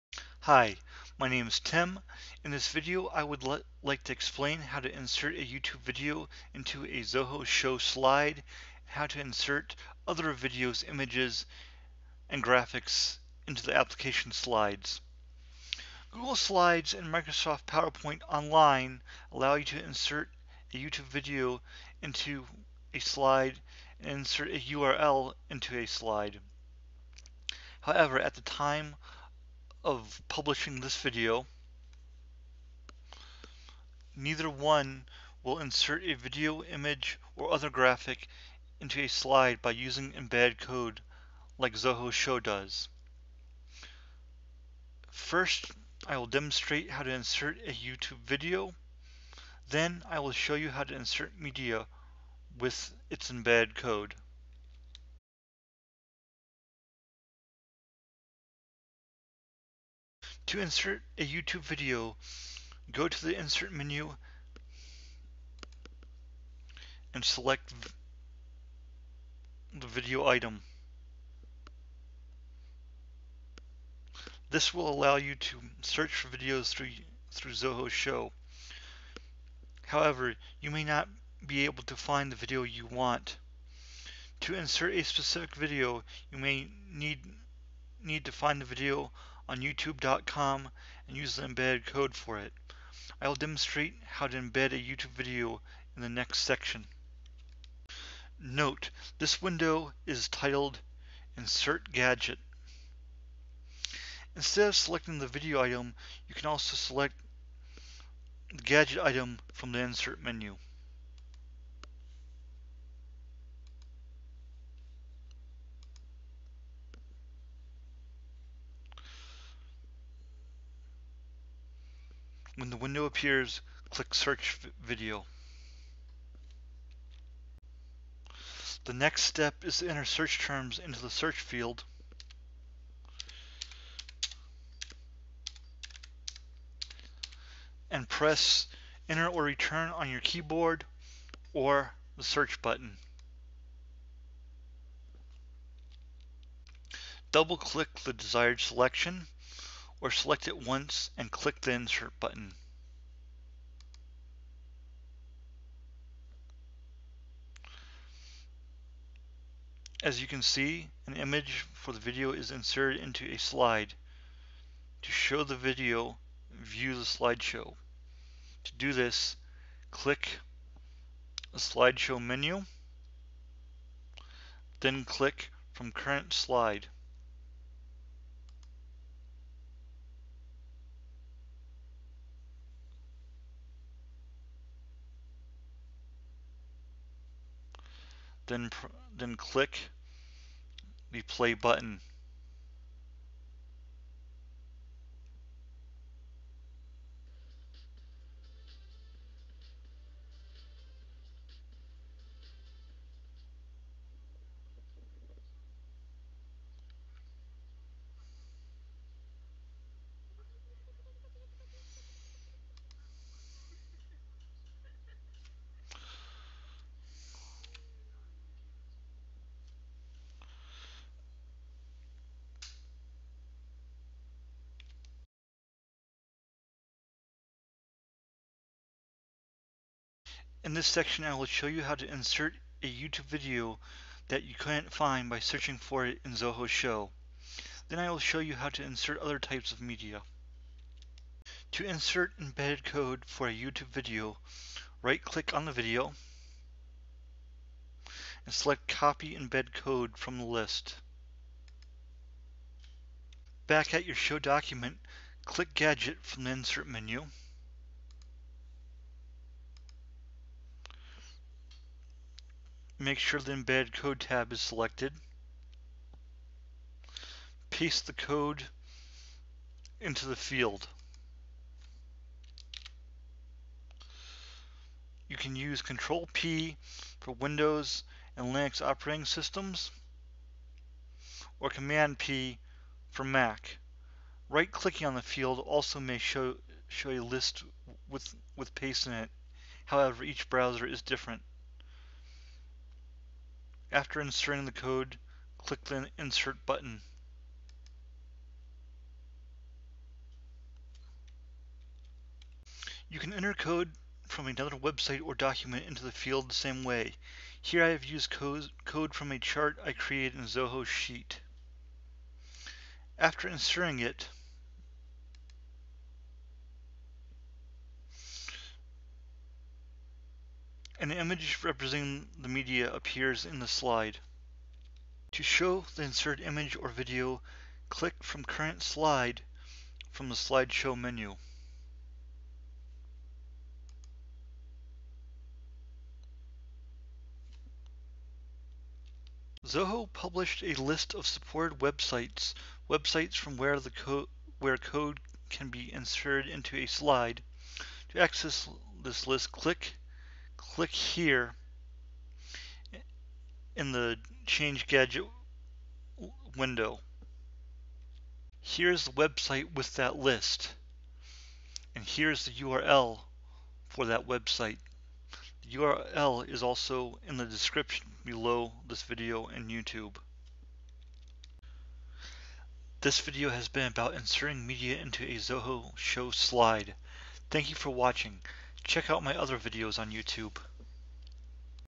Zoho Show - Inserting videos and other media into slides
This video demonstrates how to insert YouTube videos and other types of videos, graphics, and images into Zoho Show Slides. It covers searching for YouTube videos in Show and inserting media by using its embed code.

There is a list of Websites that Show supports embed code from. This list was in the video. Here is its URL: